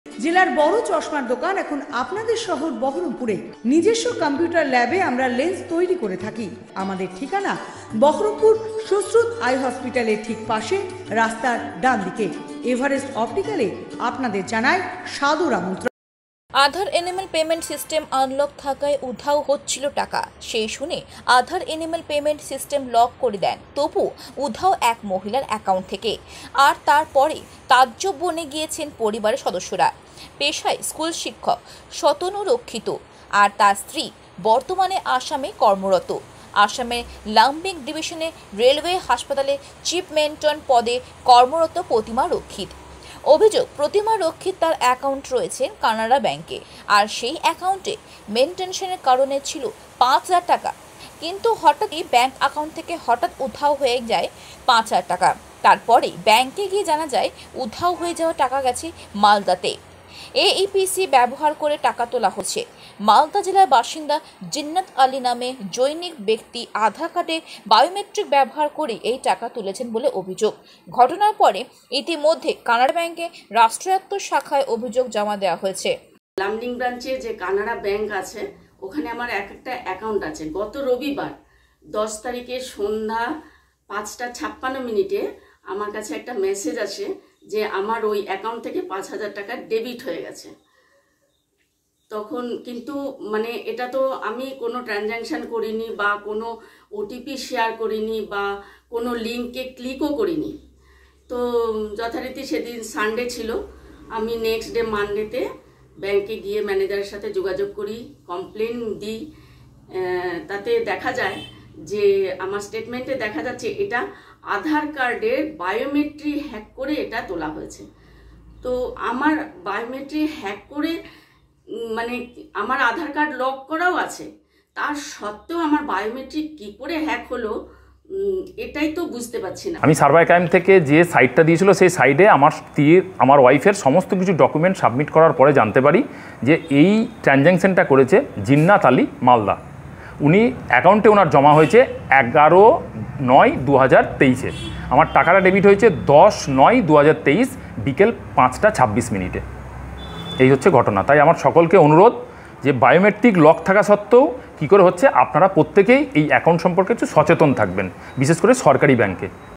Aadhaar Enabled Payment System: আধার এনেবেল পেমেন্ট সিস্টেম 'লক' তবুও উধাও টাকা
কান্দি ব্লক মোড়ের নিবাসী সুতানু রক্ষিত, তার স্ত্রী ১৯বছর ধরেই আসামে কর্মরত ।তবে এই ঘটনার পরে হতবাক পরিবারের সদস্য থেকে আত্মীয় স্বজন এমনকি পাড়া প্রতিবেশীরাও।